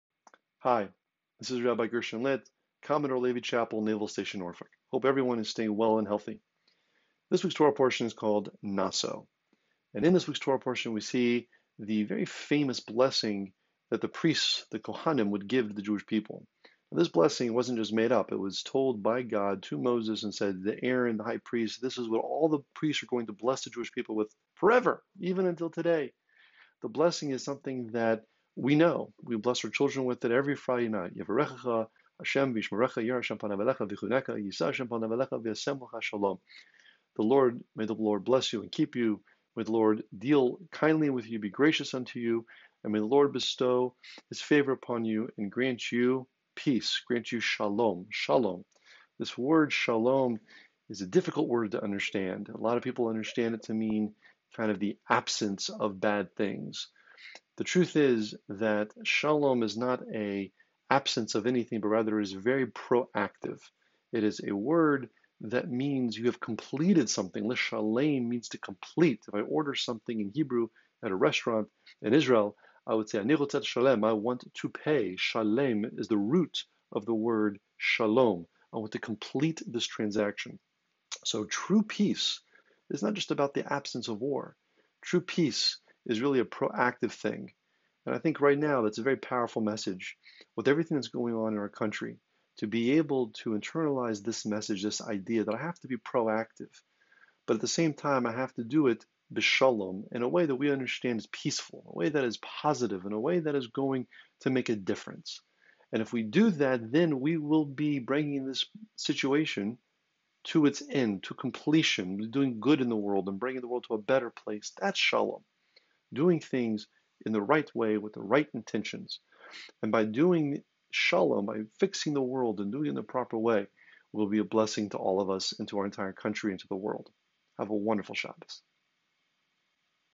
Rabbi Gershon Litt - Parshas Nasso Commodore Levy Chapel
This video is for the week of June 4th from the Commodore Levy Chapel at Naval Station Norfolk.